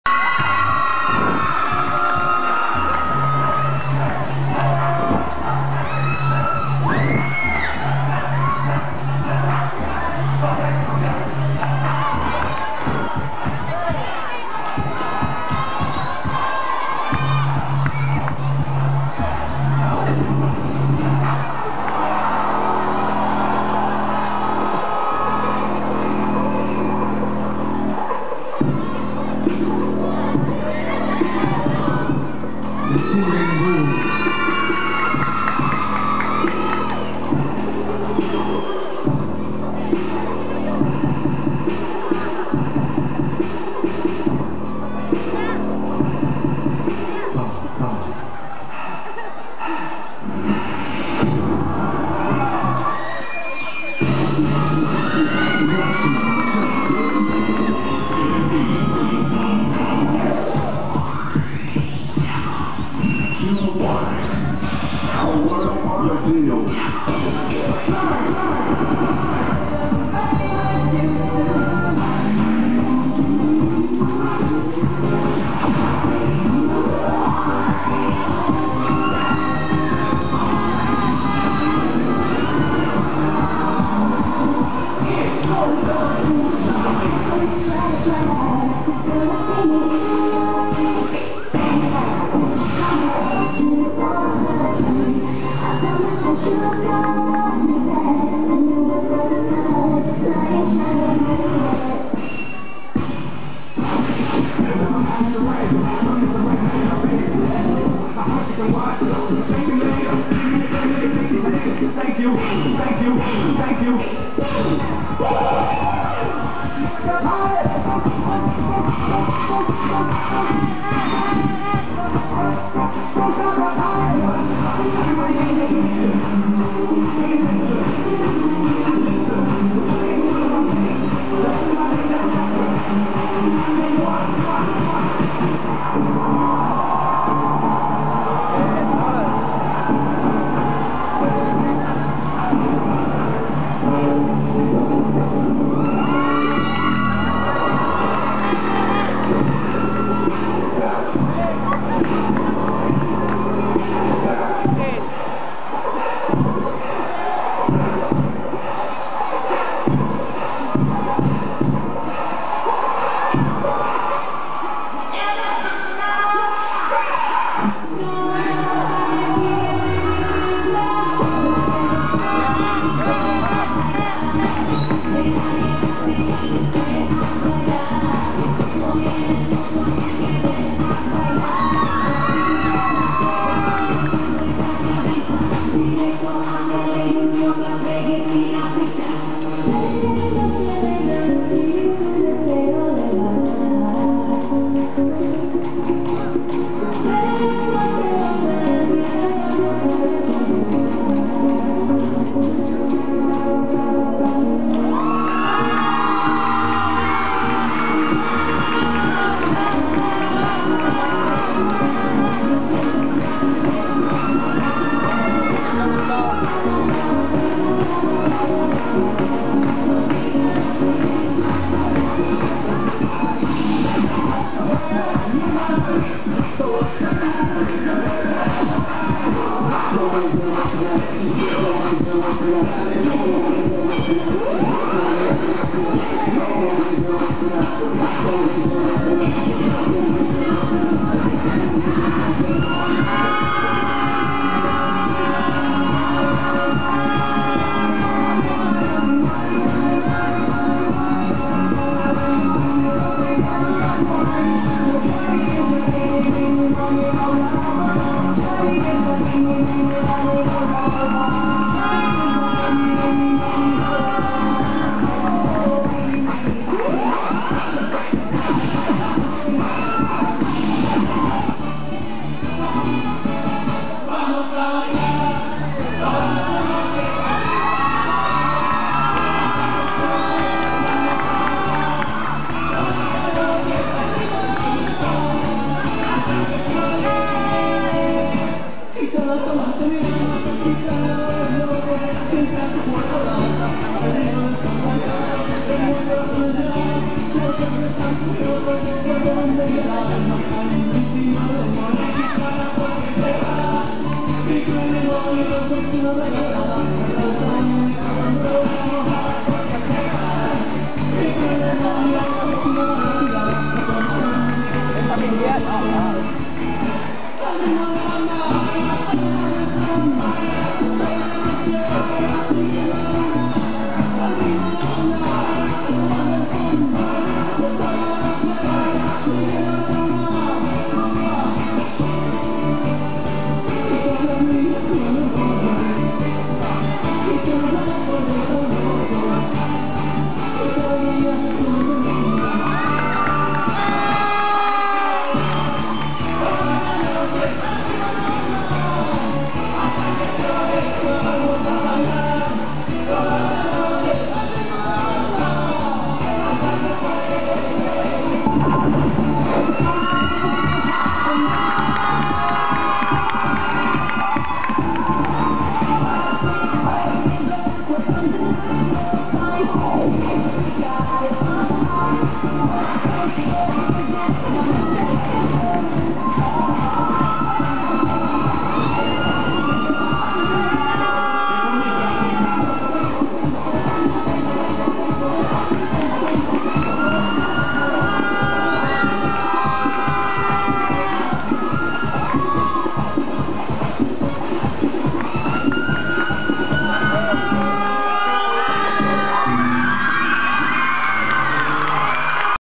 the childrens blue.avi
2 campeonato de revista gimnastica - colegio Pedro Pabon Parga - Carmen de Apicala...  THE CHILDRENS BLUE  "CAMPEONES"